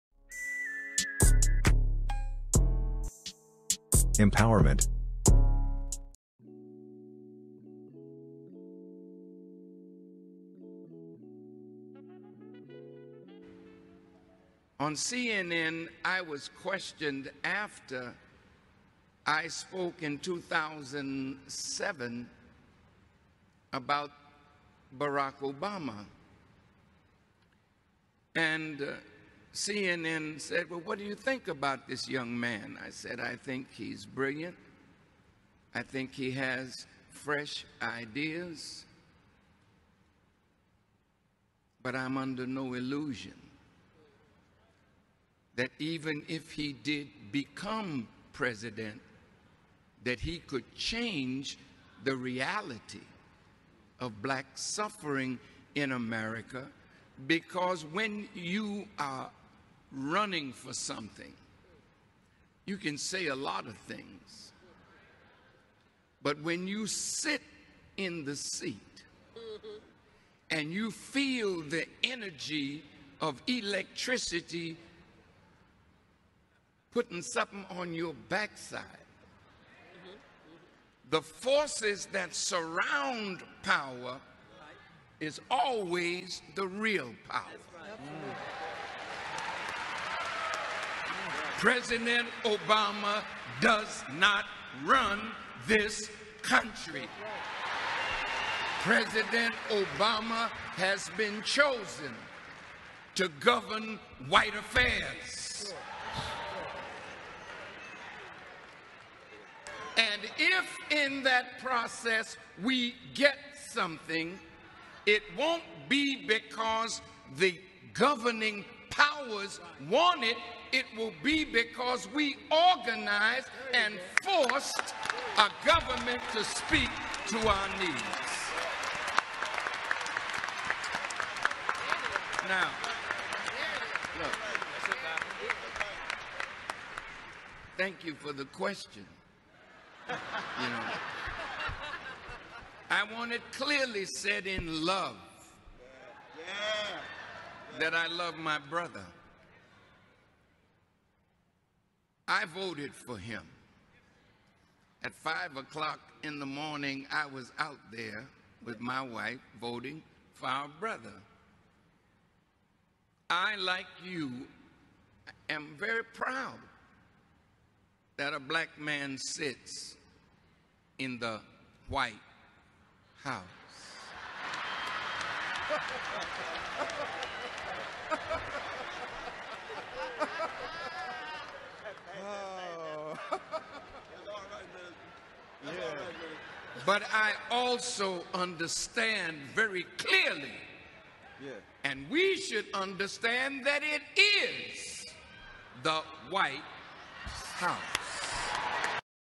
THE REAL POWER: Minister Farrakhan talk about Obama - Empowerment (JPN Television)
// The honorable Minister Louis Farrakhan explain who is Barack Obama in the WHITE House